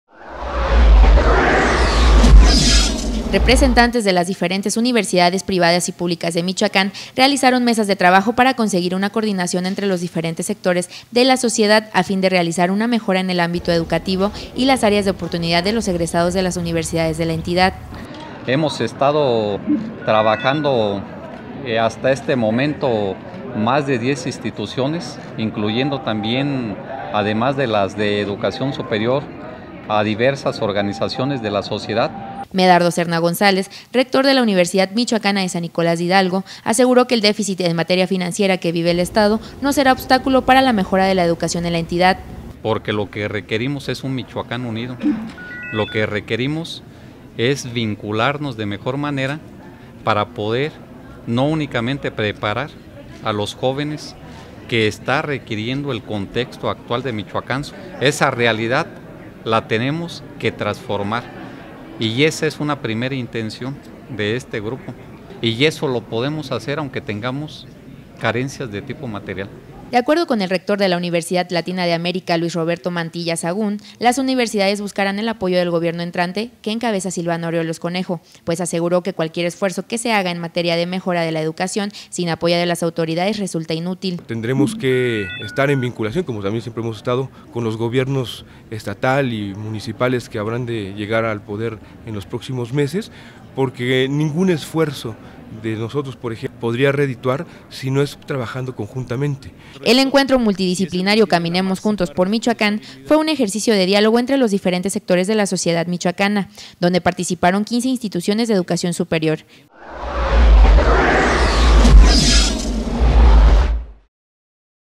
Déficit económico de Michoacán no será impedimento para mejora educativa
Morelia, Mich.- Representantes de las diferentes universidades privadas y públicas de Michoacán realizaron mesas de trabajo para conseguir una coordinación entre los diferentes sectores de la sociedad a fin de realizar una mejora en el ámbito educativo y las áreas de oportunidad de los egresados de las universidades de la entidad.